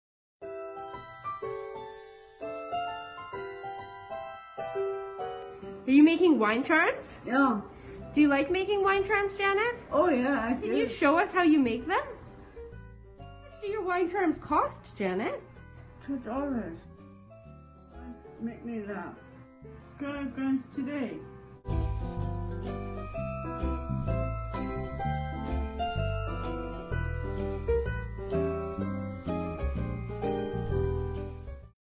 Week of Support: Janet's Handmade Wine Charms
Janet loves making products with charms and beads...Her wine charms make for the perfect stocking stuffer and come in many themes, holiday, nature, initials etc...Never mix your wine glass with someone else again with these charms!
Get your wine charms ordered today! And support Janet's wonderful product!